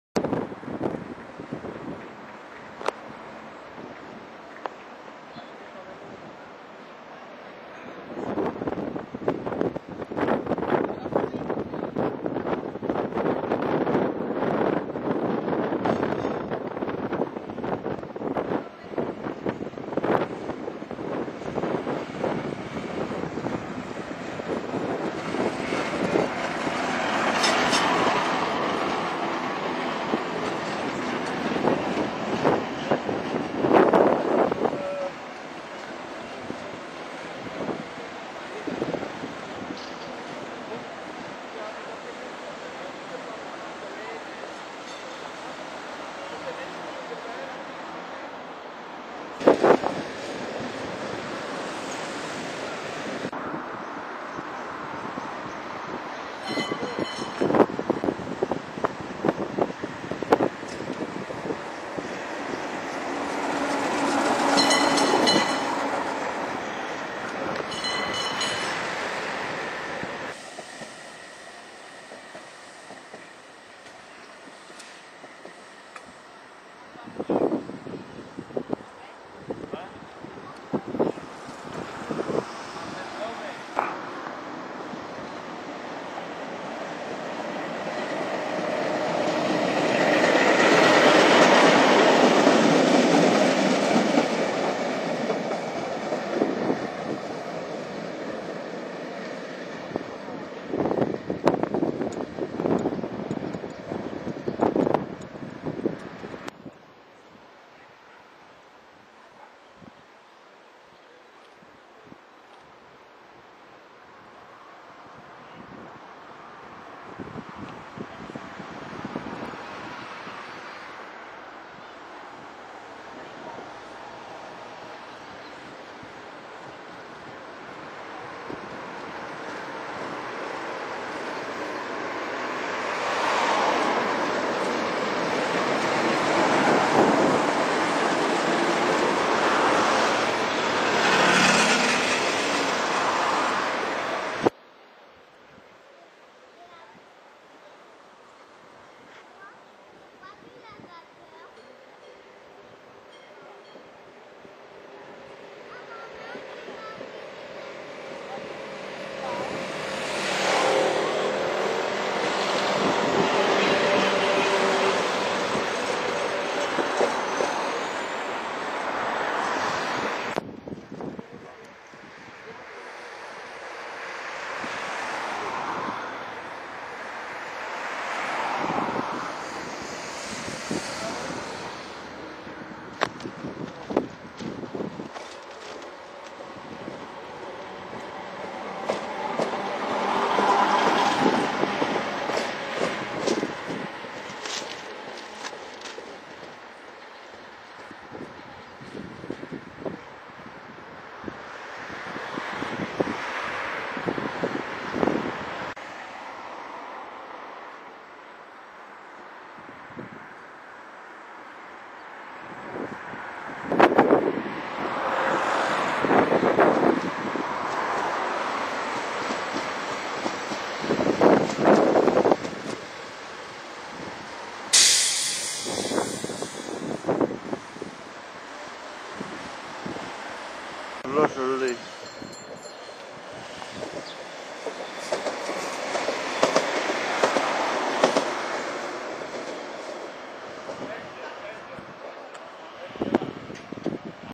125 ans kusttram tram côte belge a westende
defiler de tram a la côte belge  pour les 125 ans du kusttram a westende vers la ville d'ostende